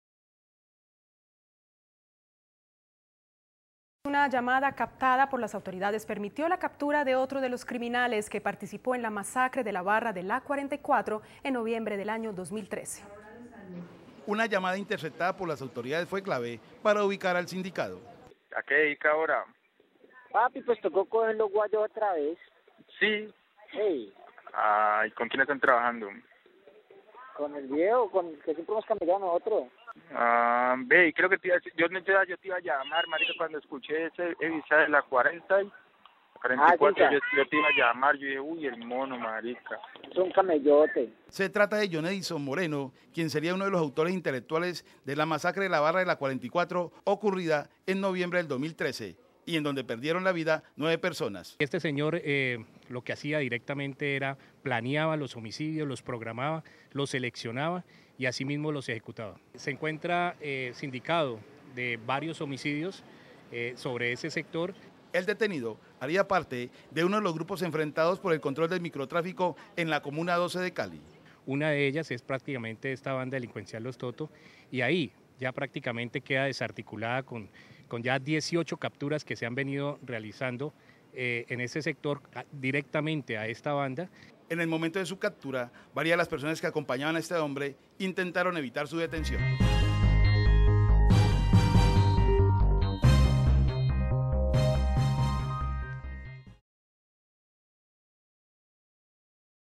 Febrero 16 de 2015 Cayó presunto responsable de la masacre en la Barra La 44
La Policía Metropolitana confirmó en la mañana de este lunes la captura de alias 'Moreno' o 'Jota', quien es señalado de ser el presunto responsable intelectual de la masacre de La Barra de la 44, ocurrida en noviembre de 2013.